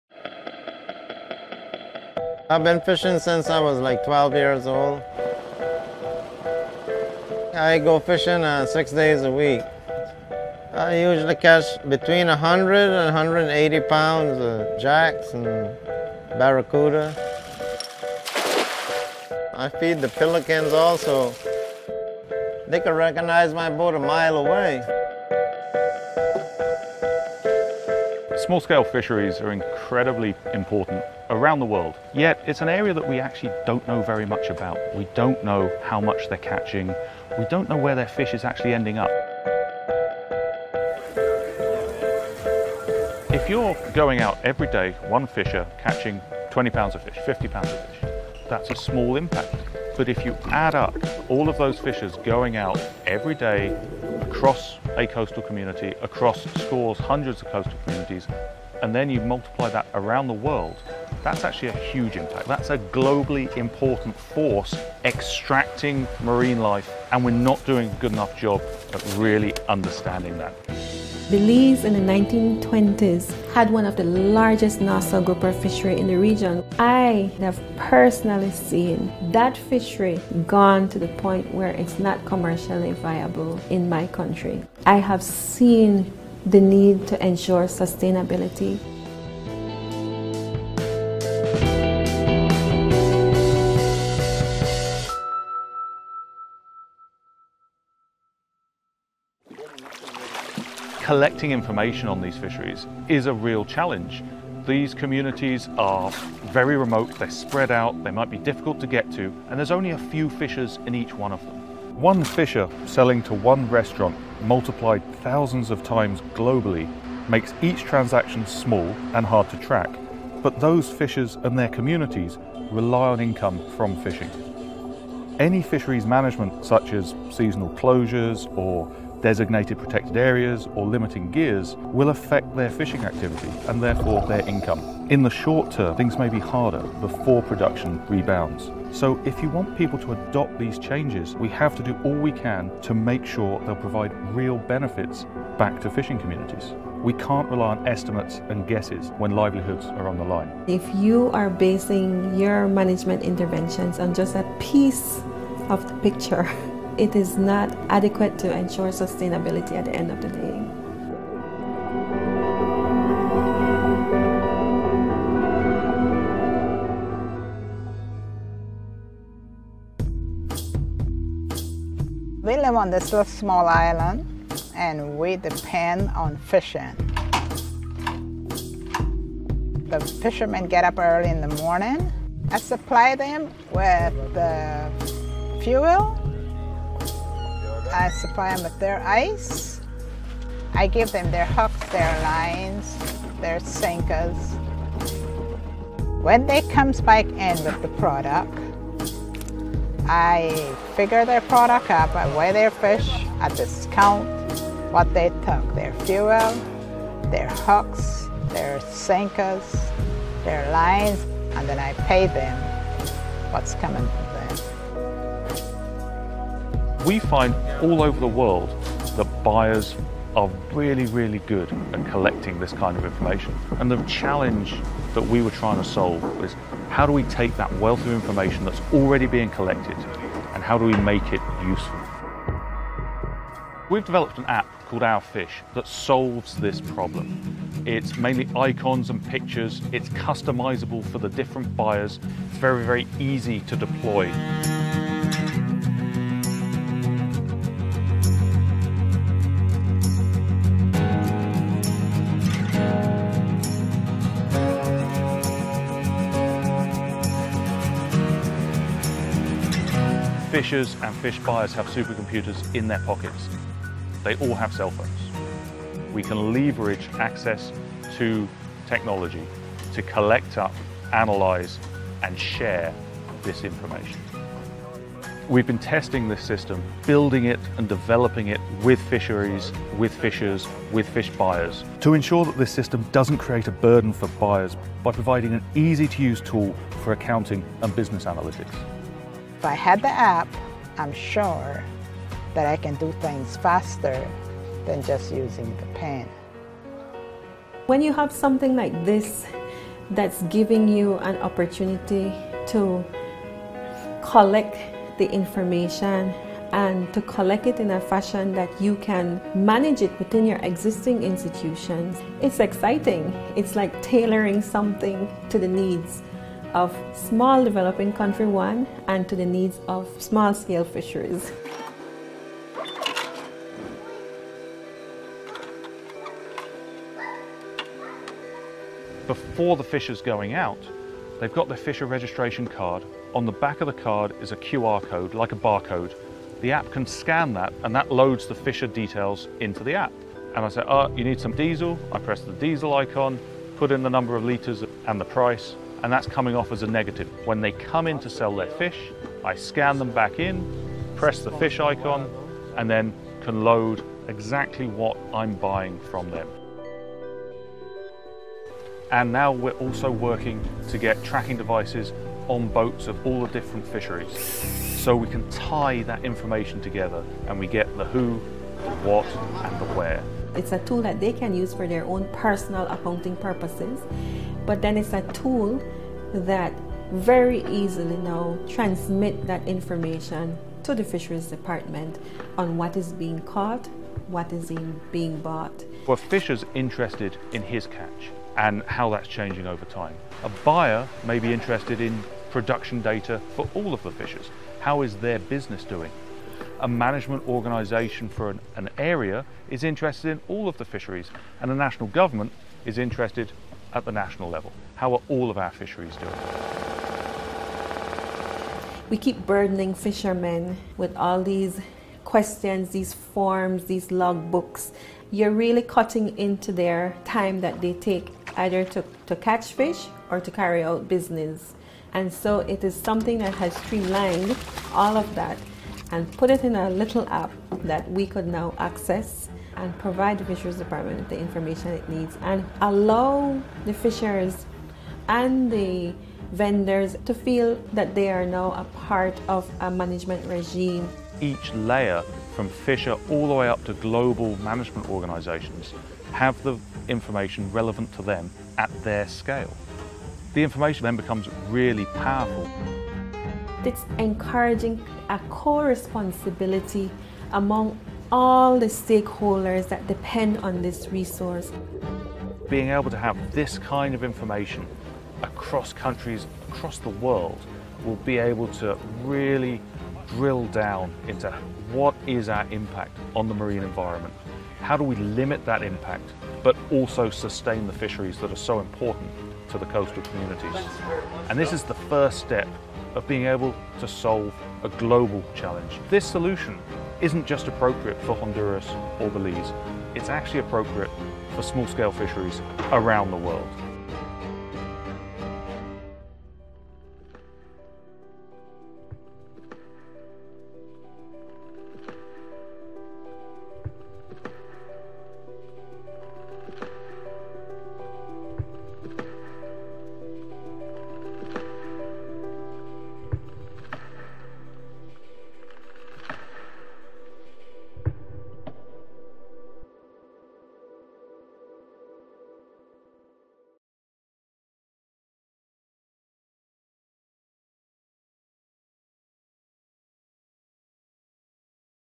The fisheries of Honduras and Belize
The Centre for Marine Ecology has a brand new way of tracking the fisheries of the Utila Cays, Bay Islands, Honduras and Caye Caulker, Belize... Check out this great piece created for National Geographic TV